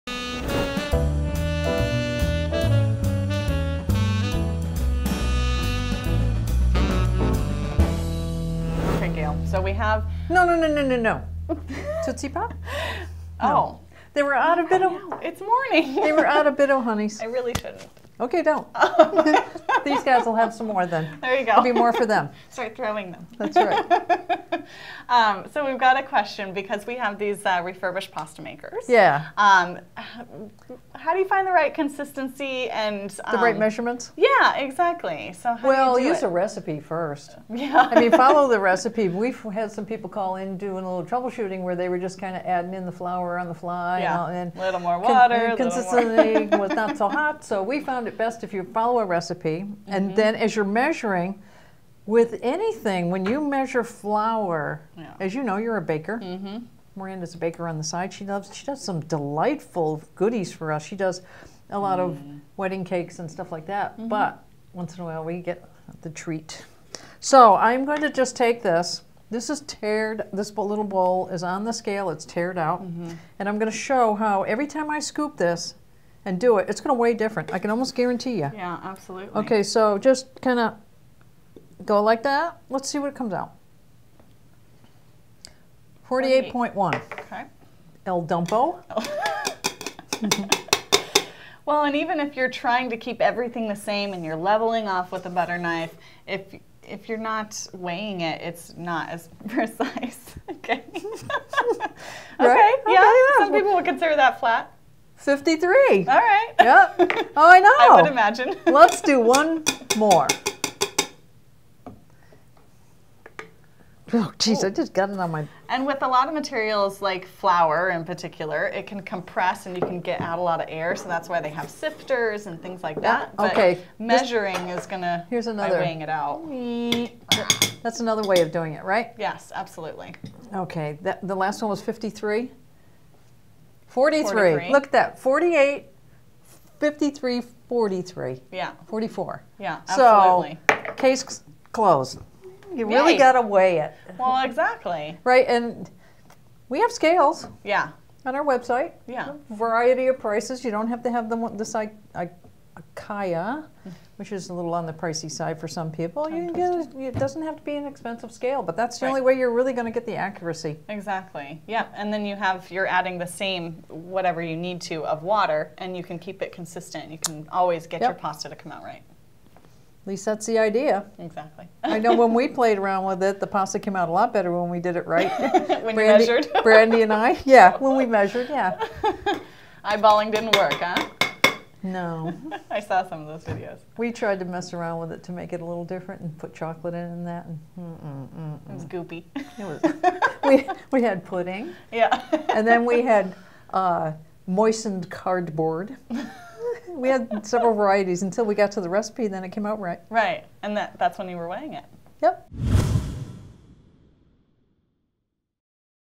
Why Weighing Ingredients Is Important | Ask Gail
Sure we are talking about weighing flour for pasta, but the same rules apply for any coffee preparation method! Watch as Gail and Miranda demonstrate why weighing your ingredients is an important step!

Have A Question For Gail? Ask Her In The Comments!

Like The Scale Gail Is Using?